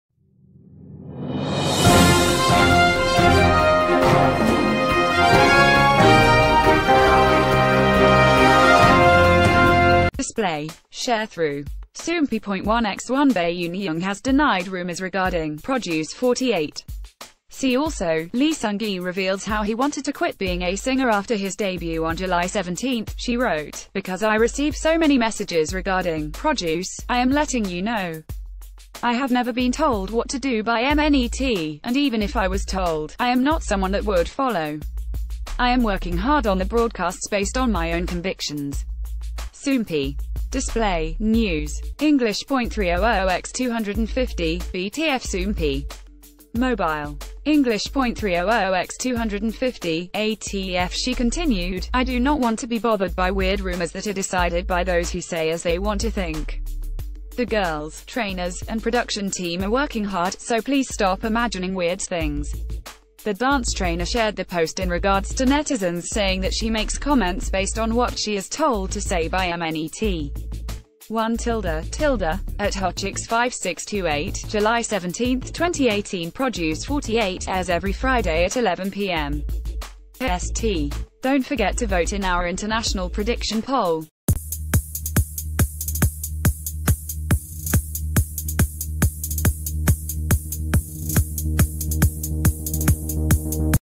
"Produce 48" Trainer Bae Yoon Jung Shuts Down Rumors Of Being Manipulated By Mnet
Display. Sharethrough. Soompi.1x1 Bae Yoon Jung has denied rumors regarding “Produce 48.” See Also: ...